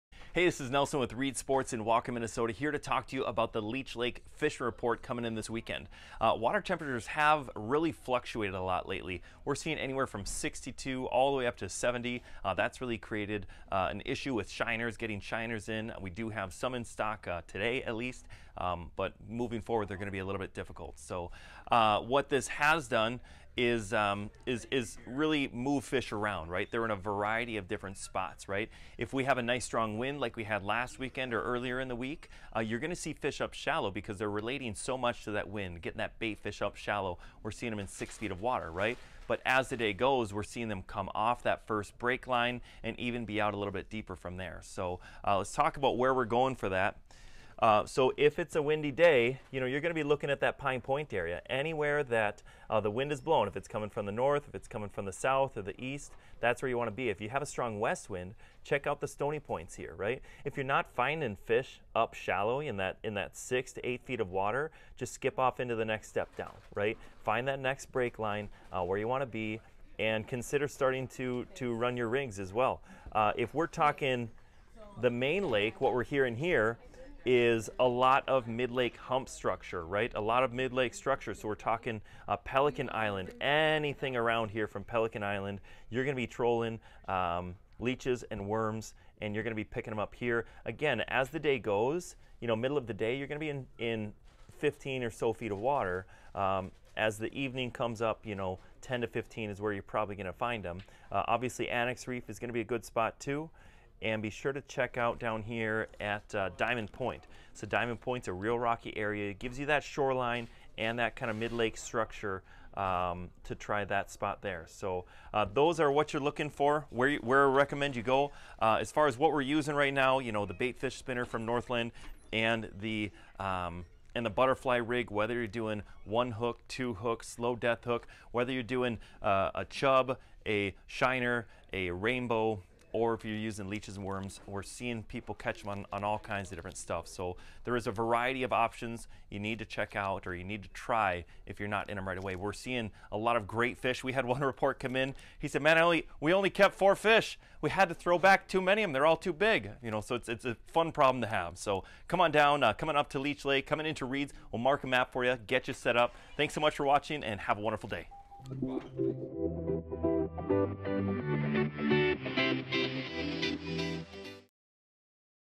Leech Lake Fishing Report 5.27.2021
Here is you Leech Lake fishing report for May 27th, 2021 from the team at Reeds Family Outdoor Outfitters!

Good luck out on the lake!